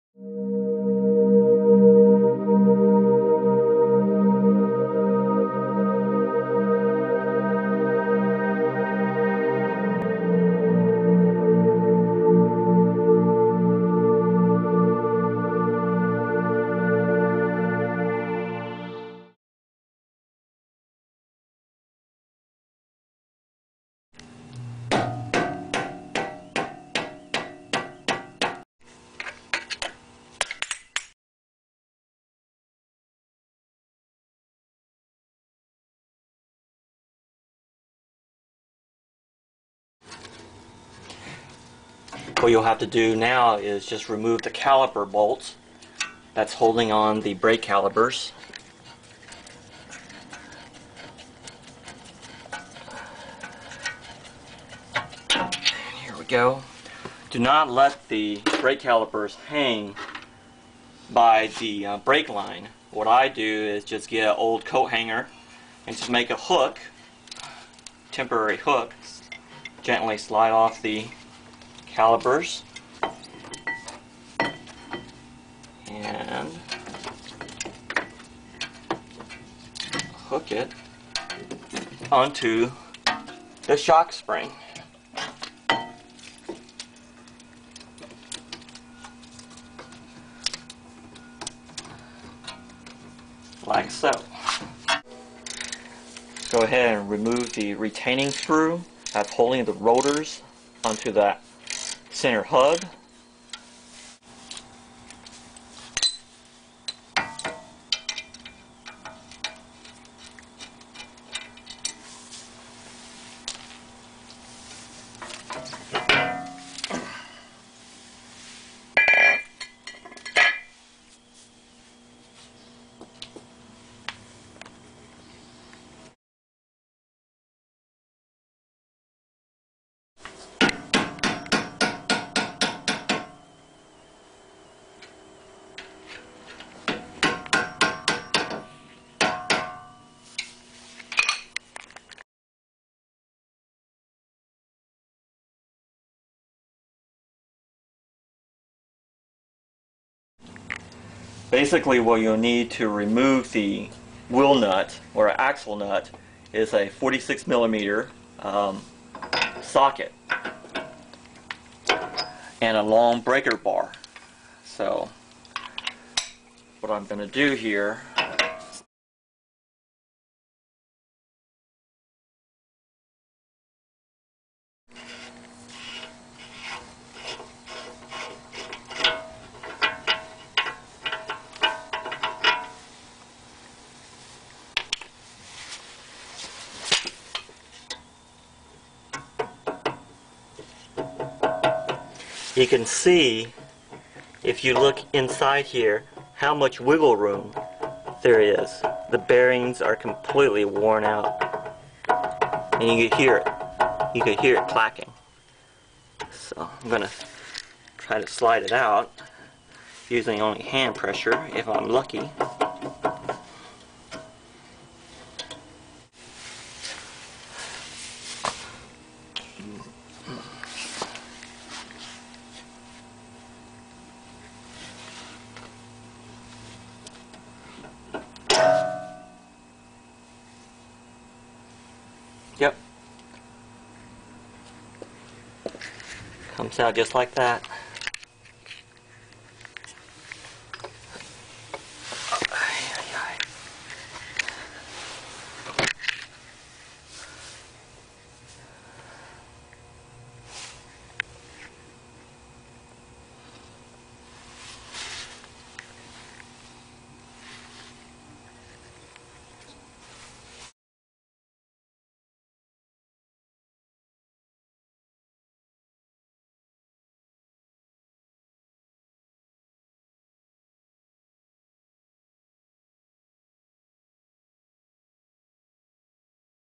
BMW Wheel Hub Bearing Replacement : DIY [ How To - Part 1 of 2 ] /// 330i (E46)
(Part 1 of 2) This video demonstrates the DIY steps on how to replace your BMW's Front Axle Hub-Bearing assembly.

(This was after I received a few Estimates on cost from different shops including a BMW dealership : between $800 to $1,250 plus tax.)

Tools needed:
Jack and JackStands
Slotted screwdriver
Hammer
8mm Hex
10mm deep socket
16mm socket
17mm deep socket
Bearing-race Installer
Ratchet extension
36mm socket
46mm socket
Torque wrench (that can hit 250 ft. lbs)
3-Jaw Puller (locking)

This DIY was performed on a 02 BMW E46 330i automatic, alpine white paint, with Sport Suspension Package, you know the drill. So if that is not your vehicle, there might be differences.